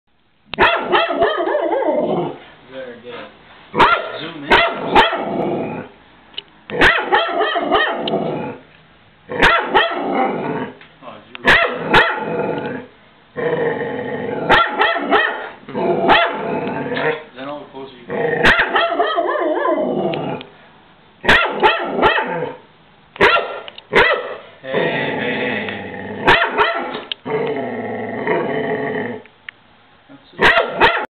my dog freakin out over the goodyear blimp
my dog blodie barking at the goodyear blimp